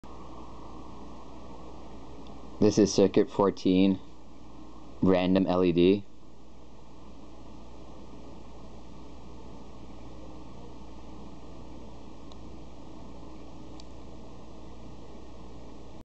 Arduino: Shift Register + LEDs Blink in Random order
leds_random
Circuit 14: Shift Register connected to LEDs
LEDs light up/blink in Random order